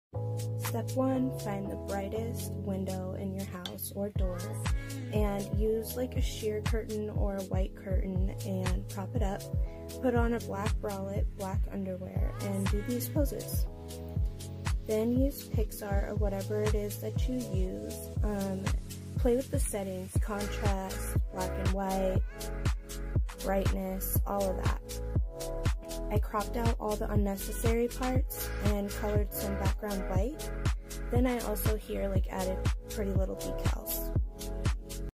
DIY MATRNITY PHOTOSHOOT
Easy matrnity photoshoot from home that anyone can do !!

Items needed:

Black bralette
Black maternity undies or shorts
White sheet or sheer or white curtain

For Cosmetics and more check out lindsayjade.com